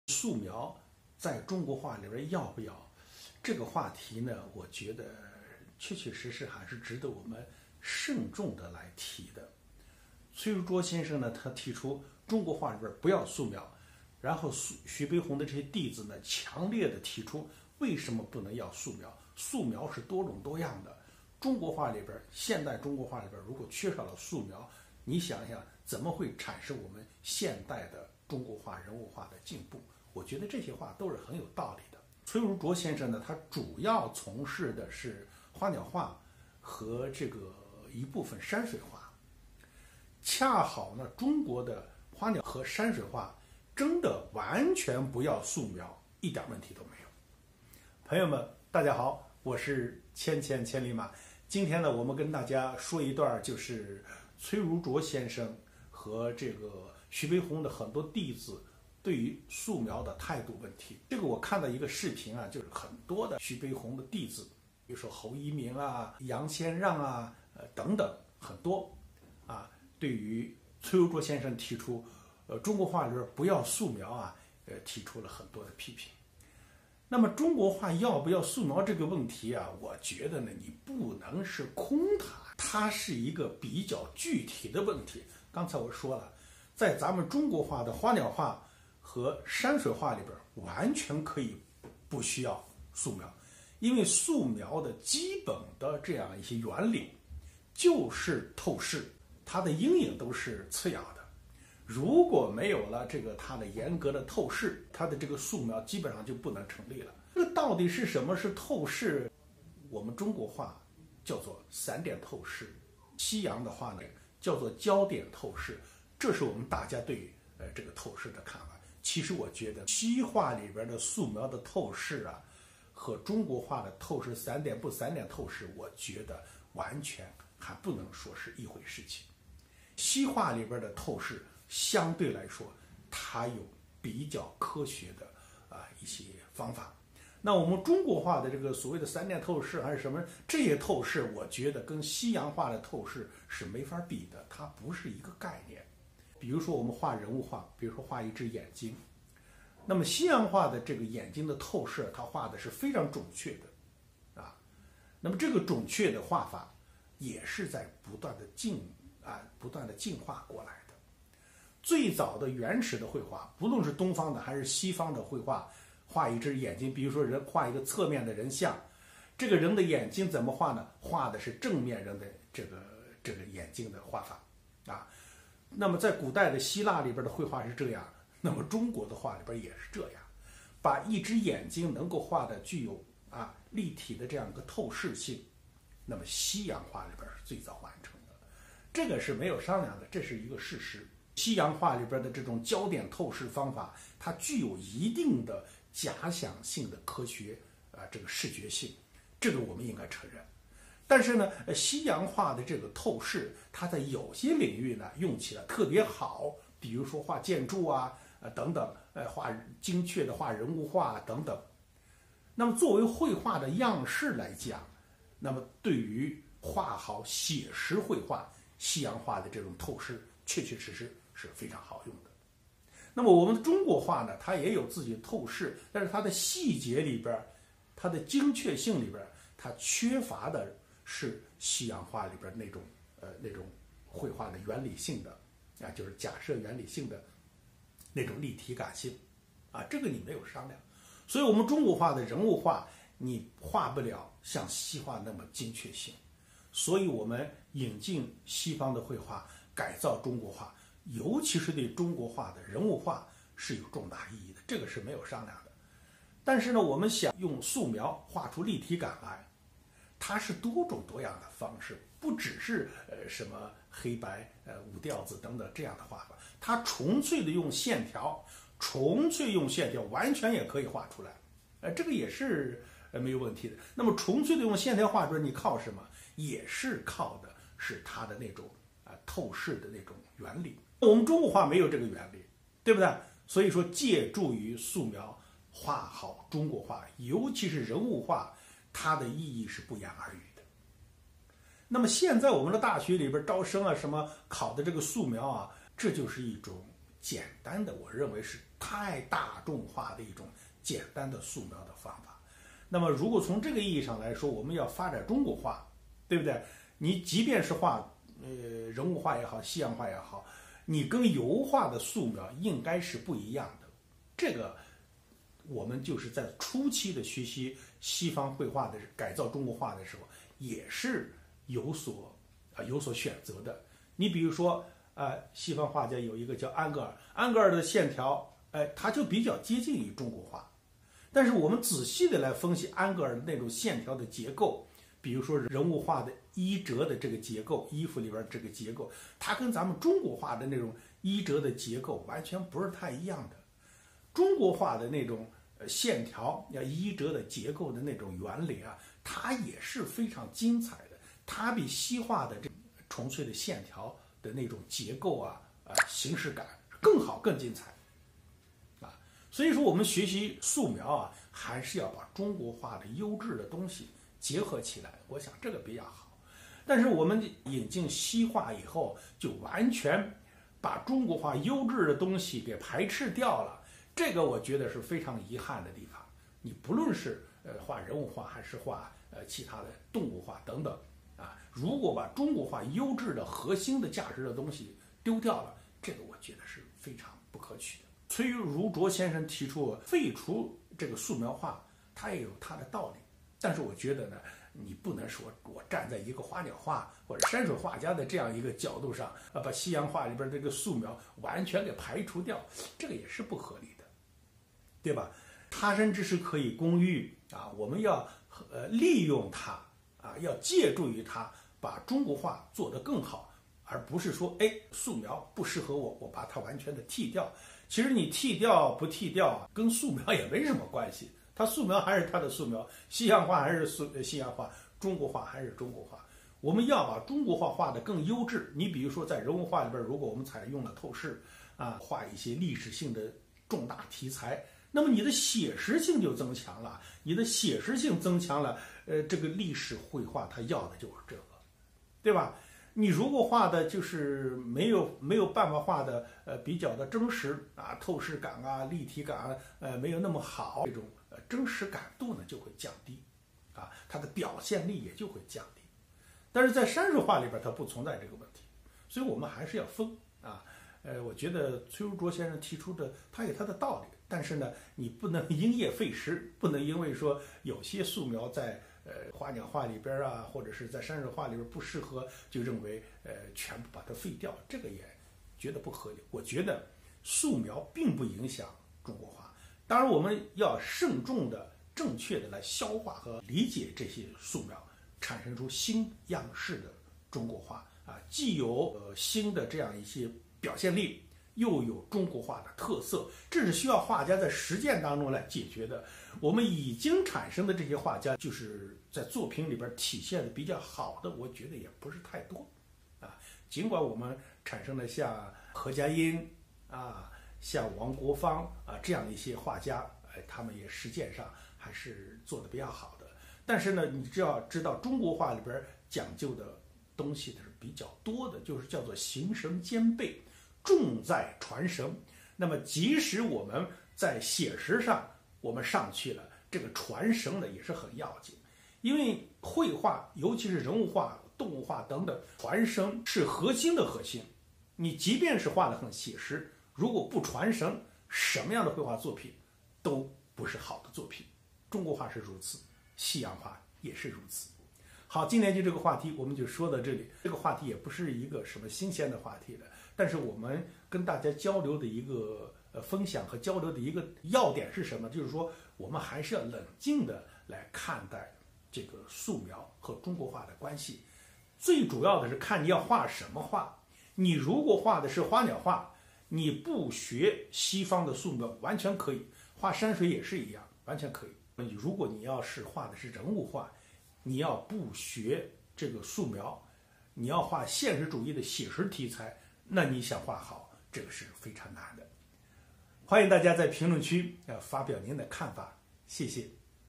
徐悲鸿的弟子为何不支持崔如琢中国画要废除素描的观点
千千千里马
认证：优质文化领域创作者
简介：艺术鉴赏，杜诗学者。专著《杜甫咏马与唐代画马》